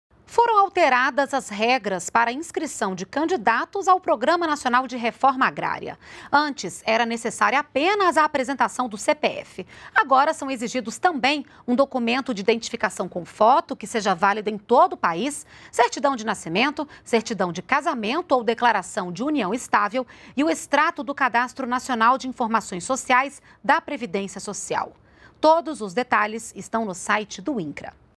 Candidatos ao Programa Nacional de Reforma Agrária devem entregar mais documentos para inscrição
NBR NOTÍCIAS - 09.03.16: Foram alteradas as regras para inscrição de candidatos ao Programa Nacional de Reforma Agrária. Antes, era necessária apenas a apresentação do CPF. Agora, são exigidos também um documento de identificação com foto que seja válido em todo o território nacional, certidão de nascimento, certidão de casamento ou declaração de união estável e o extrato do Cadastro Nacional de Informações Sociais (CNIS Cidadão), da Previdência Social. Todas as informações estão no site incra.gov.br